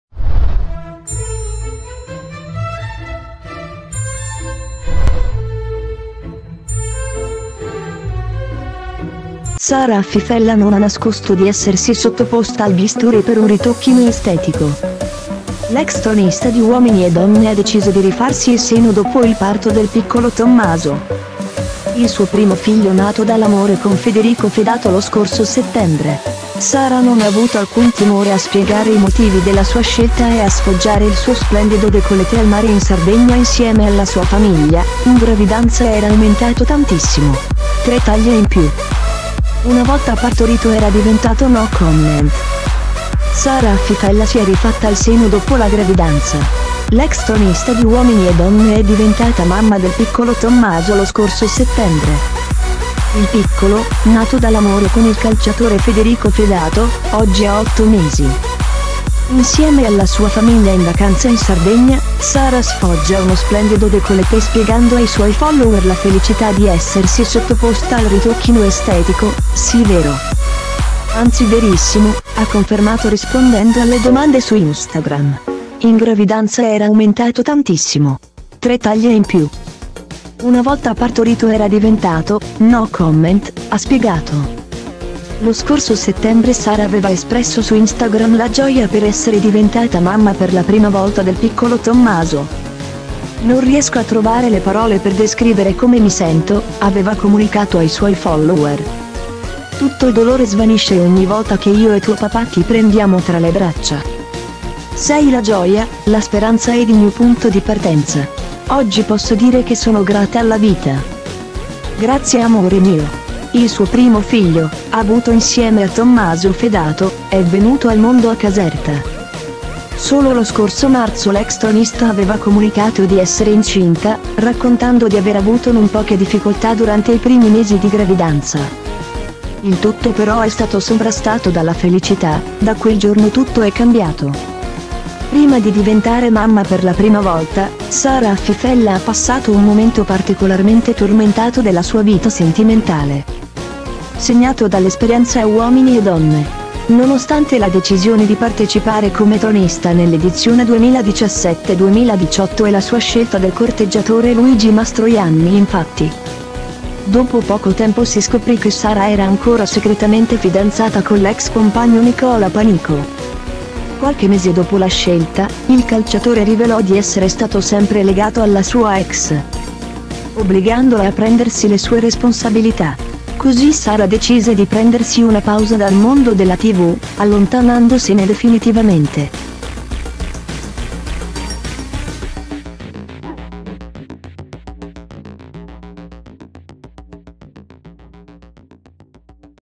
Sara Affi Fella si è rifatta seno: 'Dopo il parto era...no comment'
Sara Affi Fella non ha nascosto di essersi sottoposta al bisturi per un ritocchino estetico.L'ex tronista di Uomini e Donne ha deciso di rifarsi il seno dopo il parto del piccolo Tommaso,il suo primo figlio nato dall'amore con Federico Fedato lo scorso settembre. Sara non ha avuto alcun timore a spiegare i motivi della sua scelta e a sfoggiare il suo splendido décolleté al mare in Sardegna insieme alla sua famiglia: 'In gravidanza era aumentato tantissimo ! Tre taglie in più.